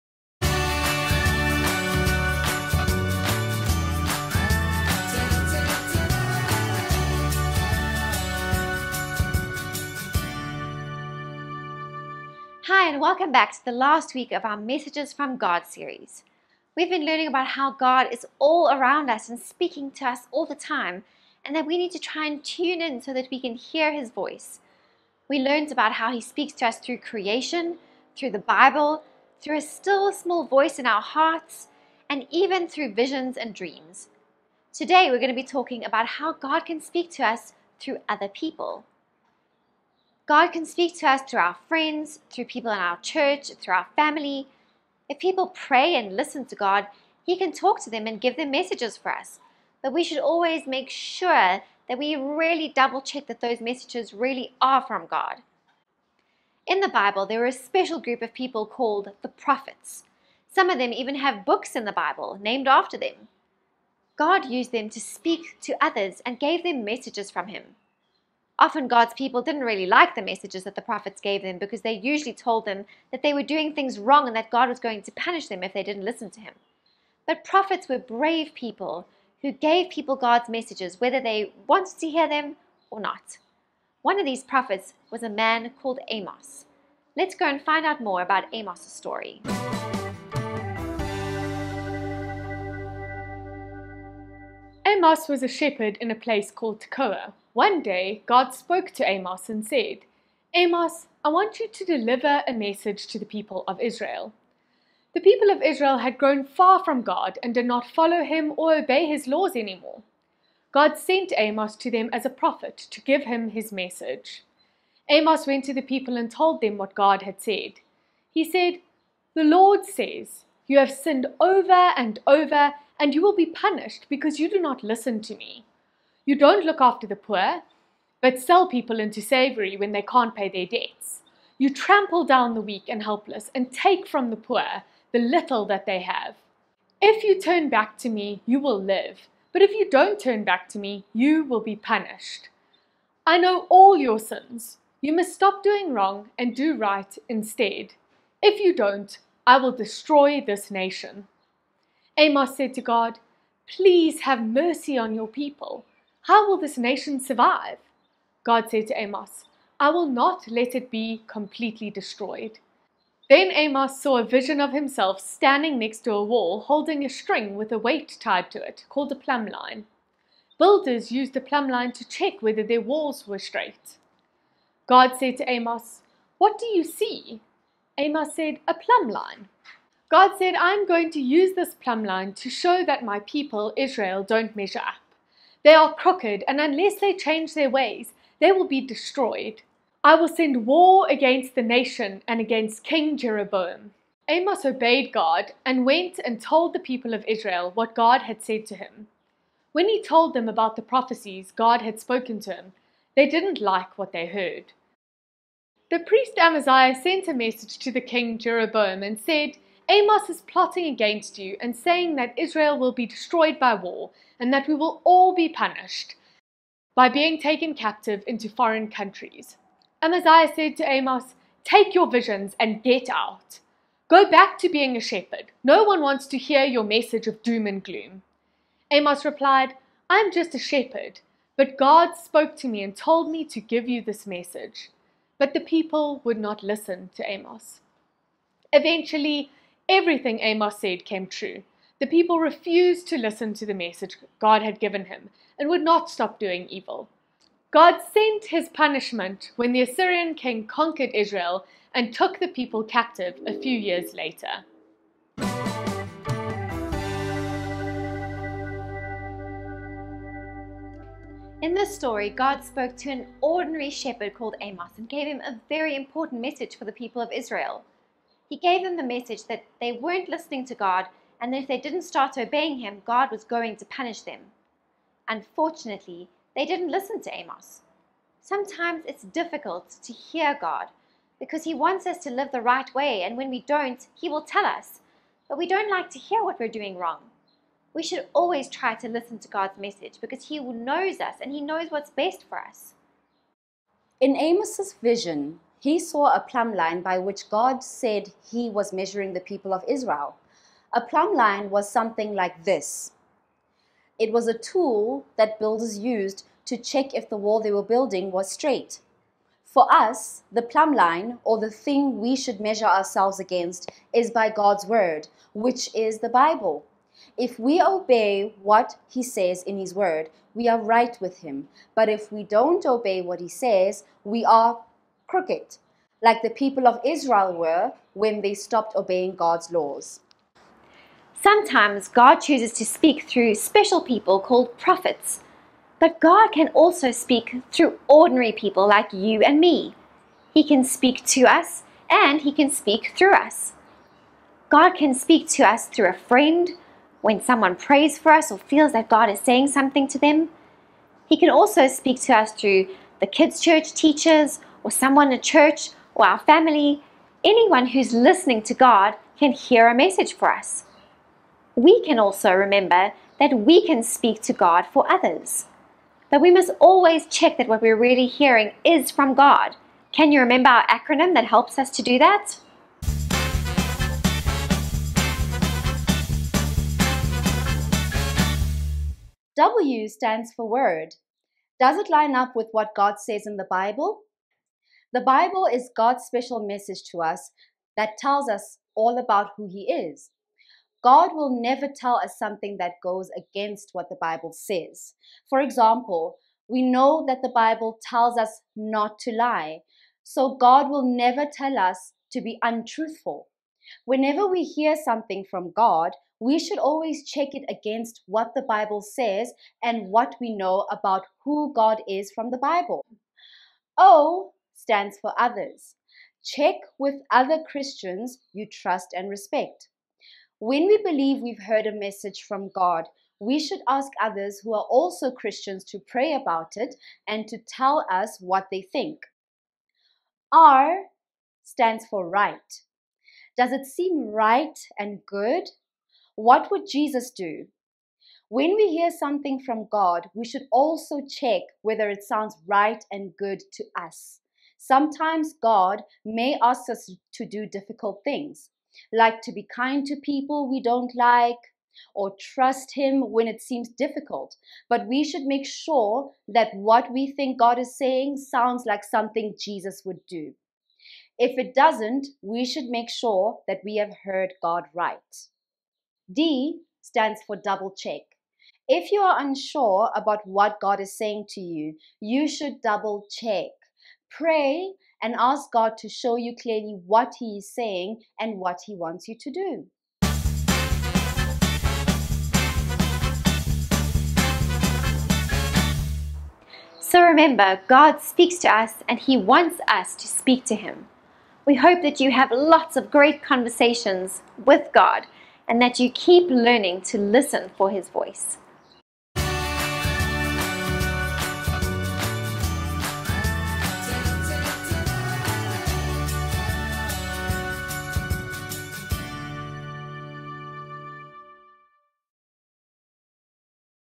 Messages from God 5
A Bible story for primary schoolers about prophets (particularly Amos) and how God can speak to us through others.